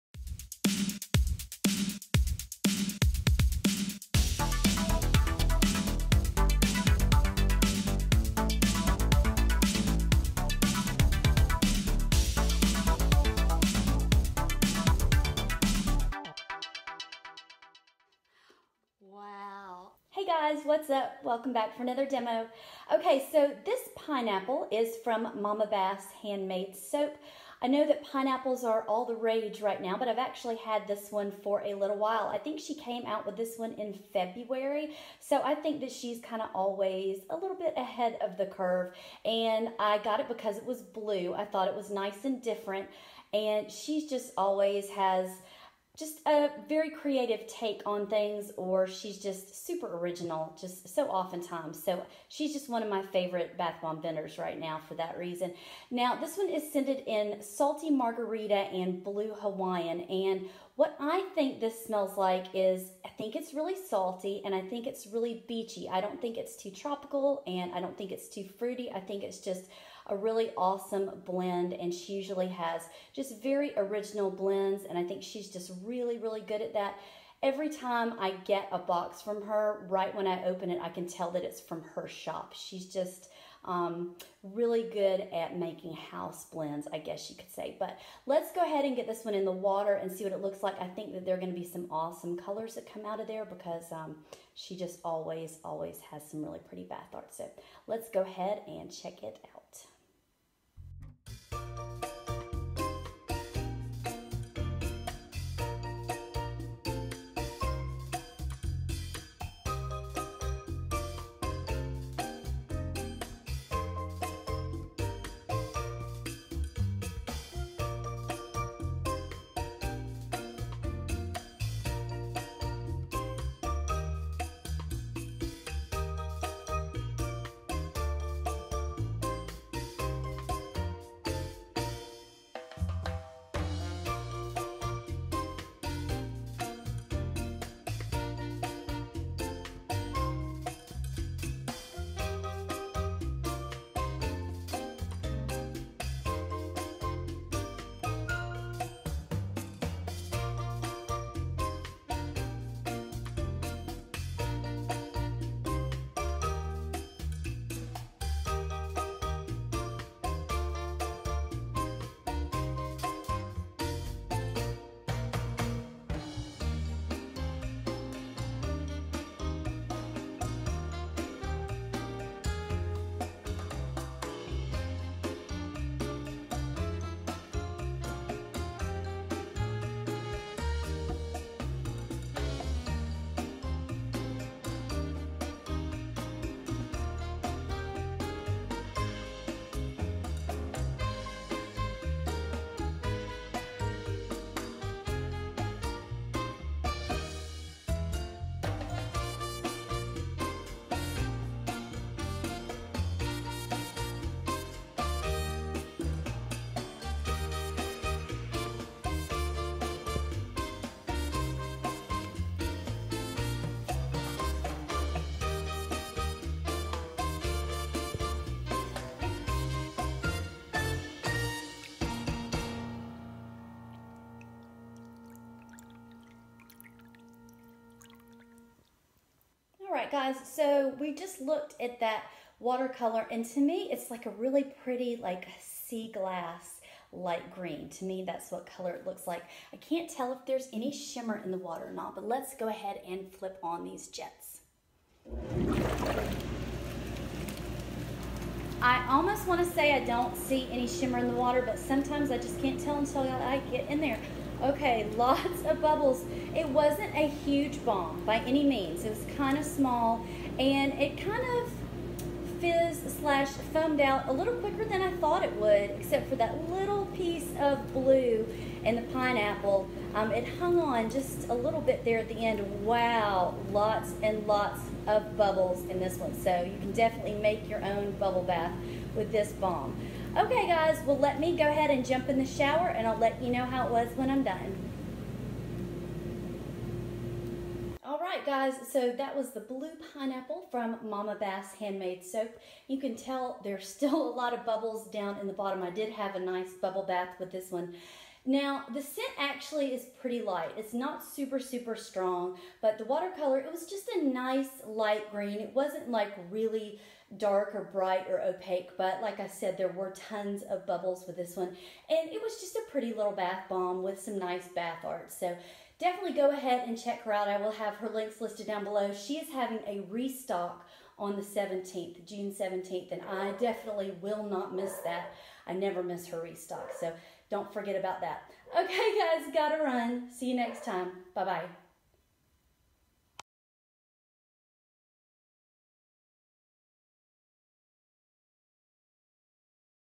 MAMA BASS HANDMADE SOAP - "Blue Pineapple" Bath Bomb Demo & Review in Jetted Tub *Summer 2018*
I hope you enjoyed today’s demo & review of the “Blue Pineapple” bath bomb in my jetted tub from MAMA BASS HANDMADE SOAP. There is always some nice bath art and never any cleanup needed w/ her bath bombs and the bubbles are an added plus! Go check her out! Don’t forget about her restock on June 17th! Links are listed below.

*MAMA BASS HANDMADE SOAP*